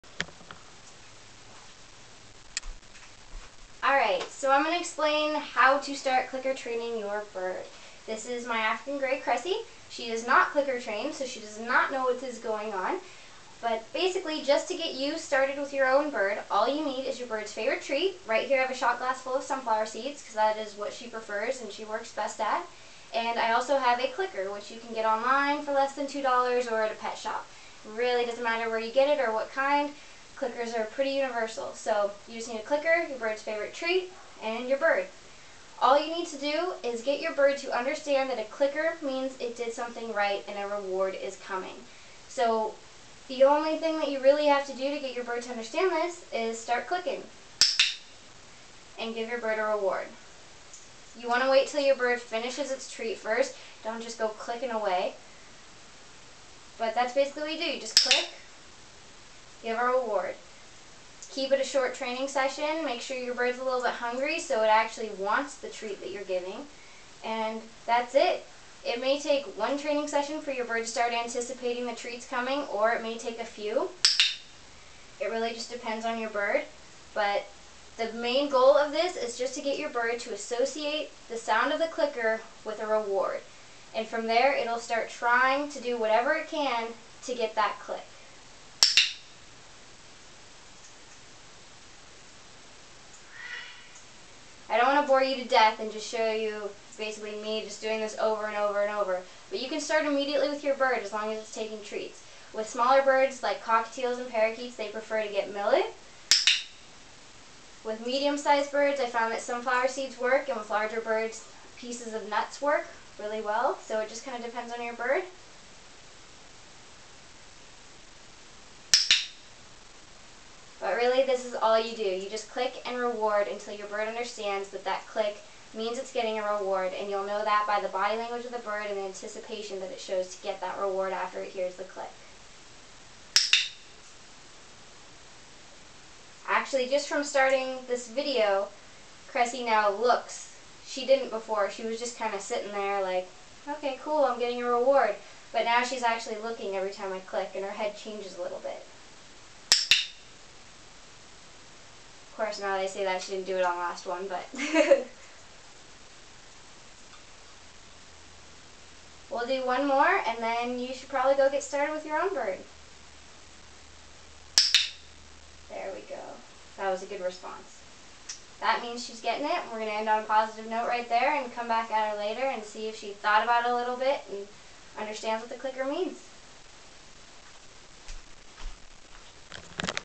Clicker Training Made Easy
Jamieleigh explains how to get started with clicker training!